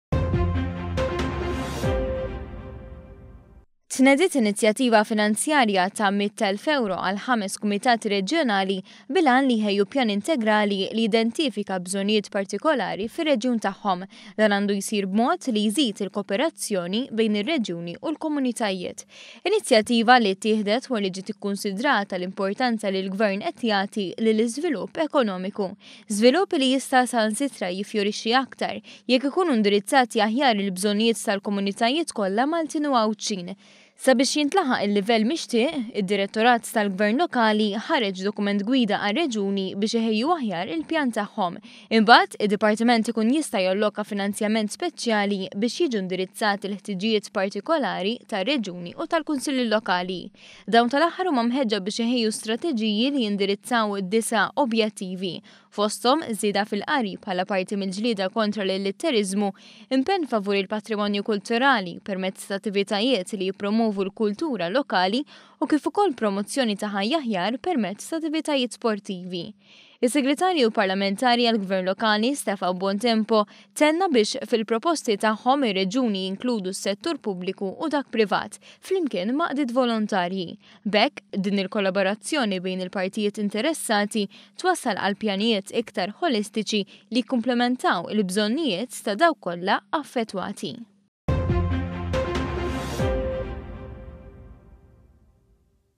€100,000 GĦAL ĦAMES KUMITATI REĠJONALI.
ONE.COM.MT - Biex leħen iċ-ċittadin jersaq dejjem eqreb lejn il-Gvern Ċentrali, tnediet nizjattiva finanzjarja ta’ mitt elf ewro li se tingħata lil ħames Kumitati Reġjonali. Dan biex sa Marzu li ġej jitħejja pjan realistiku li jidentifika l-bżonnijiet fir-reġjun, u bidliet ta’ kif dawn jistgħu jiġu implimentati fi żmien adekwat.